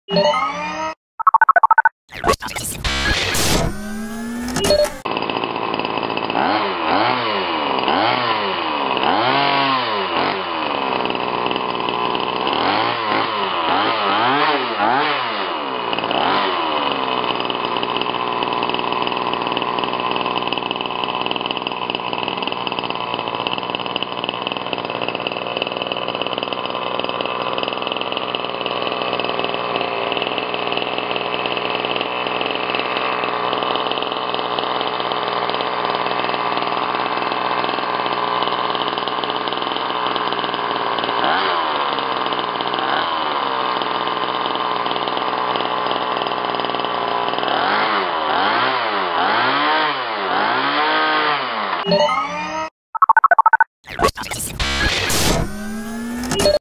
23/30cc Tunned 1:5 Scale Motor - Baja to F1 Custom Conversions
HPI Baja 1:5 scale truck to F1 custom conversion - Pre tuned muffler setup. 23/30cc Tunned 1:5 Scale Motor - Baja to F1 Custom Conversions.